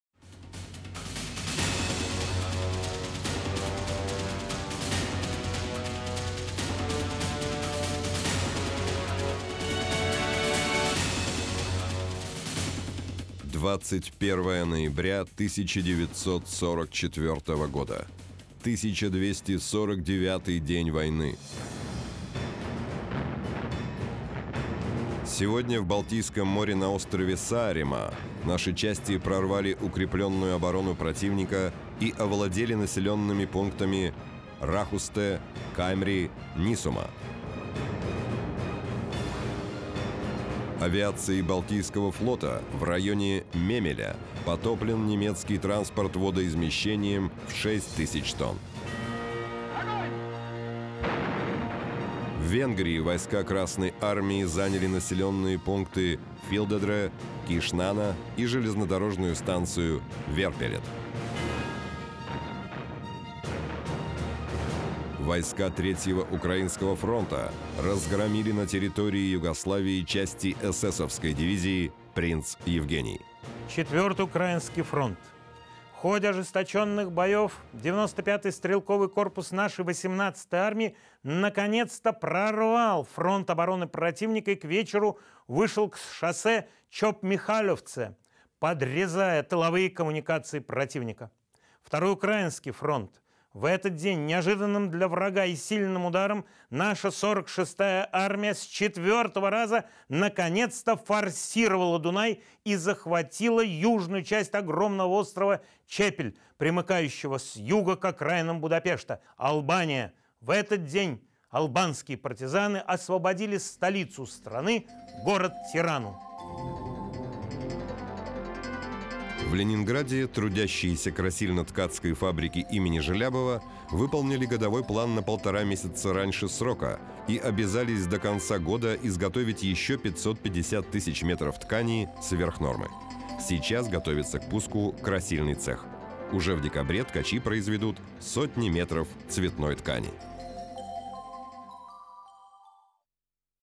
Освобождение 21 ноября 1944: на о. Сааремаа наши части прорвали укреплённую оборону противника
Шел 1249 день войны. Советские части овладели населёнными пунктами Рахусте, Каймри, Нисума.
Авиацией Балтийского флота в районе Мемеля потоплен немецкий транспорт водоизмещением в шесть тысяч тонн.